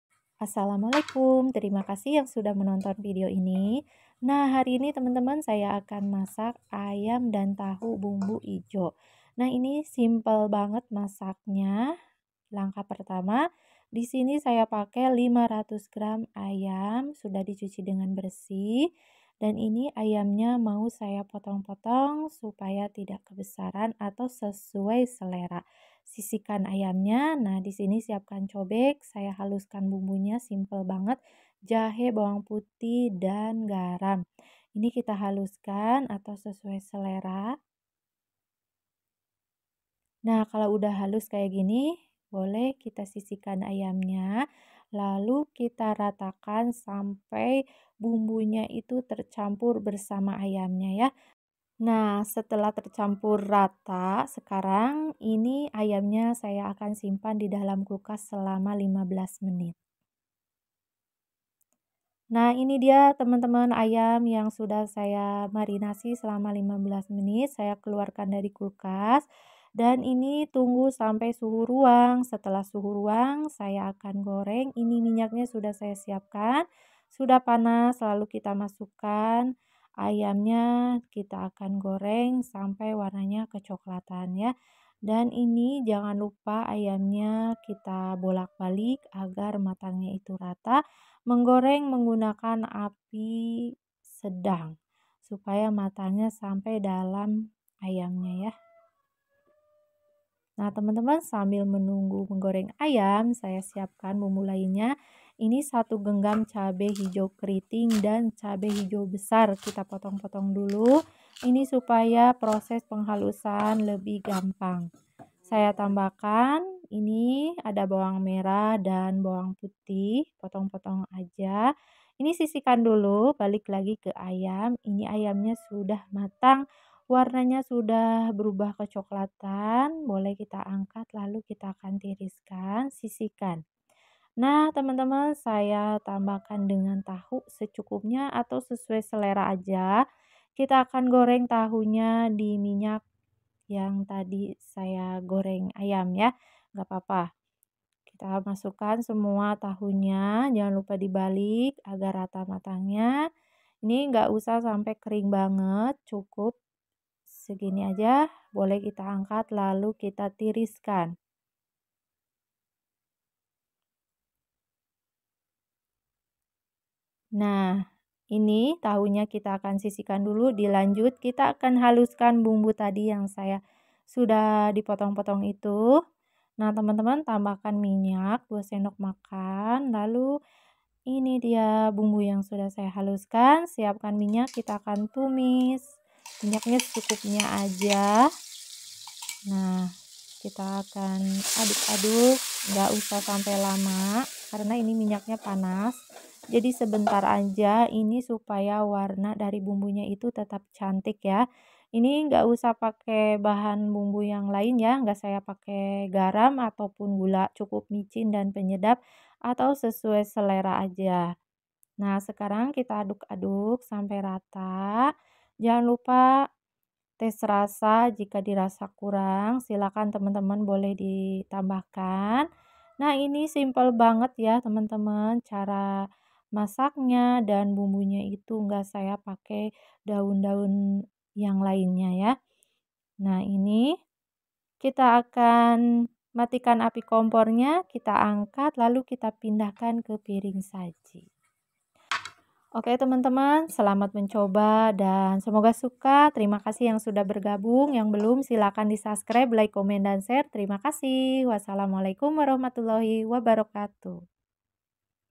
AYAM DAN TAHU DI MASAK BEGINI ENAK
Ayam dan tahu di masak dengan bumbu cabe ijo sangat enak @ani elvina

Bahan
500g sayap ayam
2 siung bawang putih
1/2 cm jahe
1 sdt garam

Tahu sesuai selera

Minyak secukupnya untuk menggoreng ayam

1 genggam cabe hijau campur
2 siung bawang putih
7 siung bawang merah
2 sdm mintak goreng

1/4 sdt micin
1 sdt penyedap rasa

Selamat mencoba semoga suka ❤️